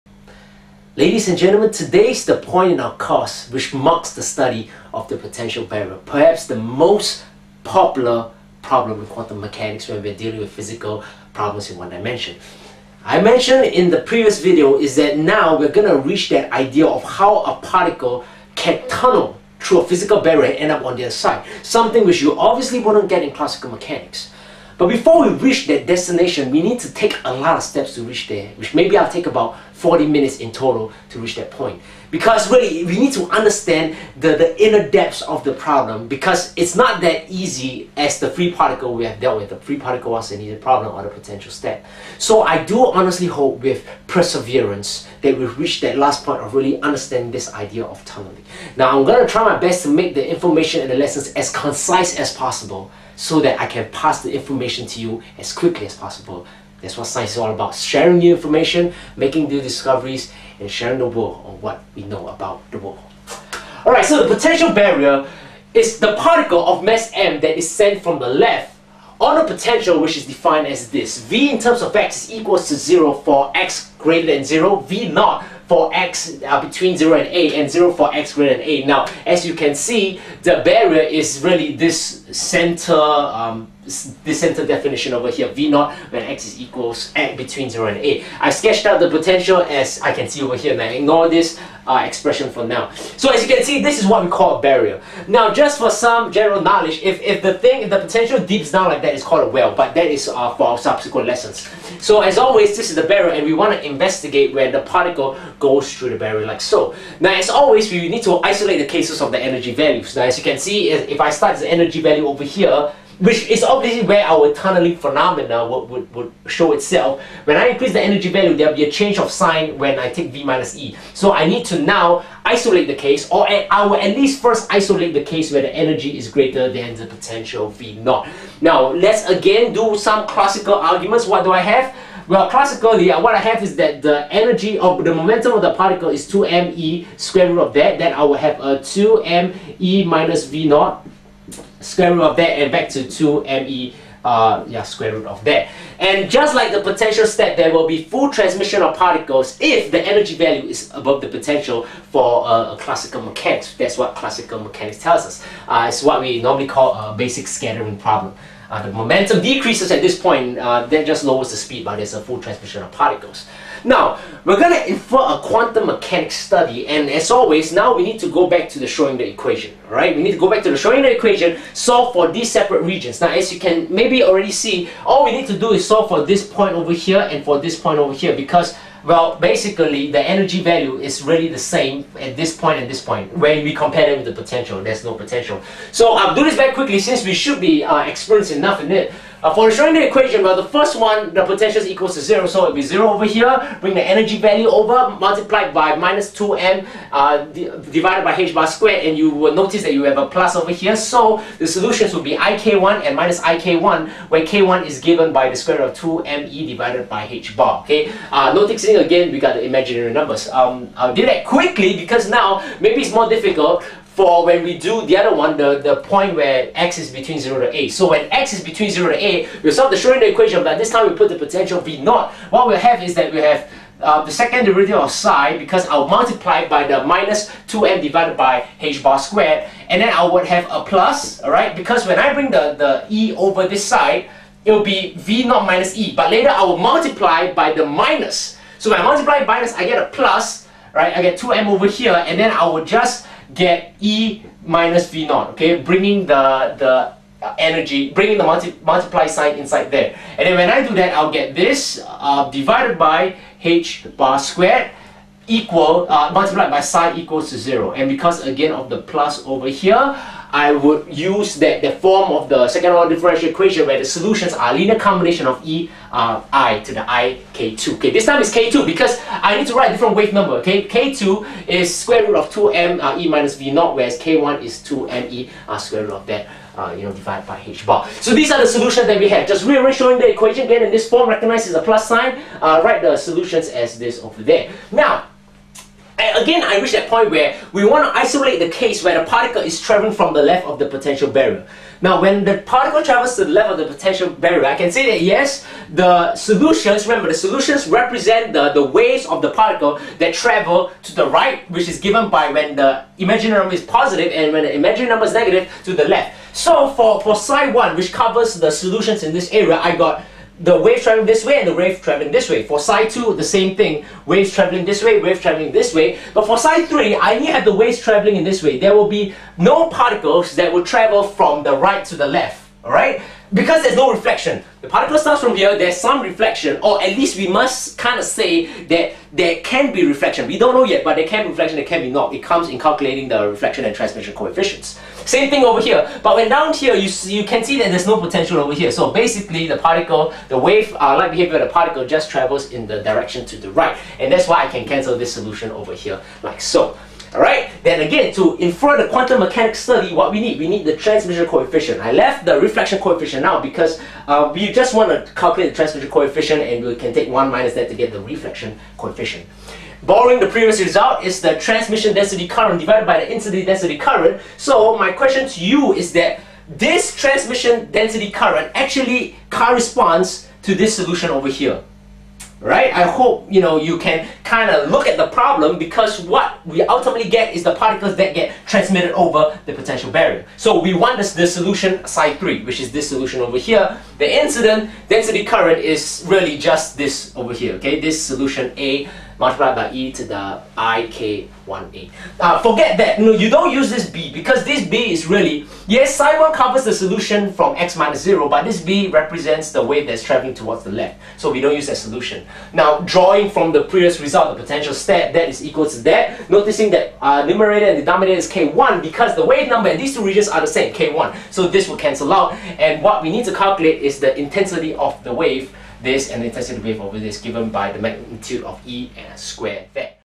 QM4.1: Potential Barrier E ≥ V - The scattering problem
The Potential Barrier, case: E ≥ V₀
We easily infer the quantum mechanical study from the treatment of the potential step. For this scattering problem where the energy of the the particle is greater than the potential, we simply solve the Schrödinger equation keeping in mind that the potential is V₀ only for the width of the barrier.